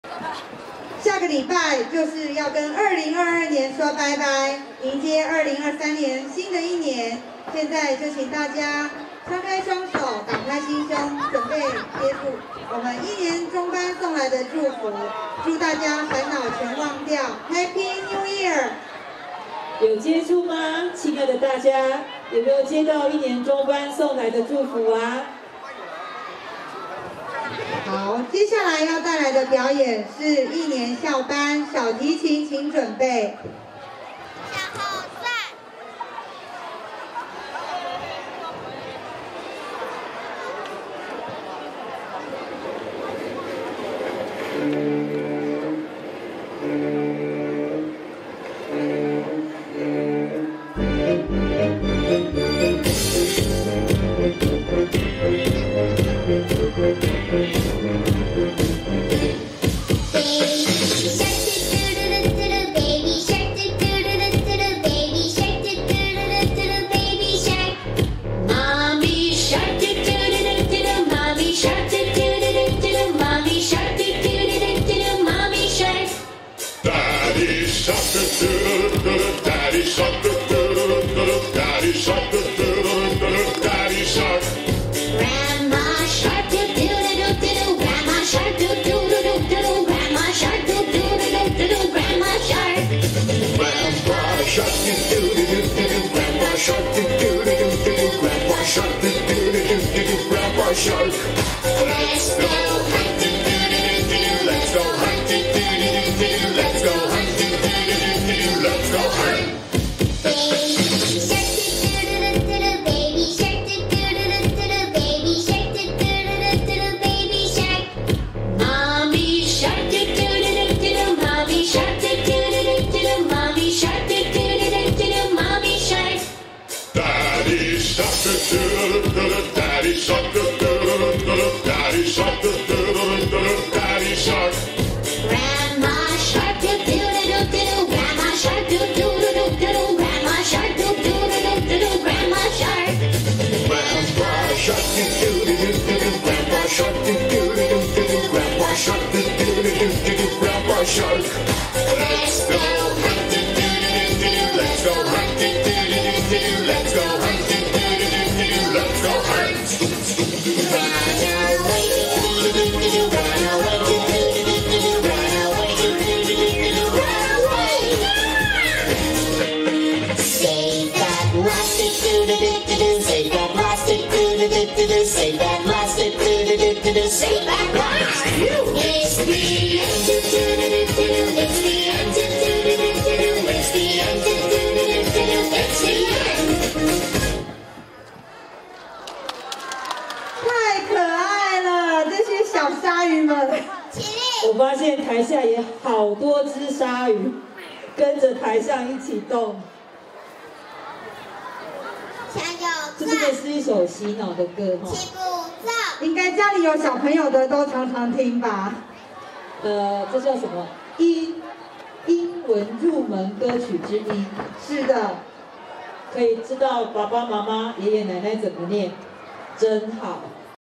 10一年孝班舞蹈表演
111學年度-光芒四射-精彩集結-宜蘭縣四結國小成果發表會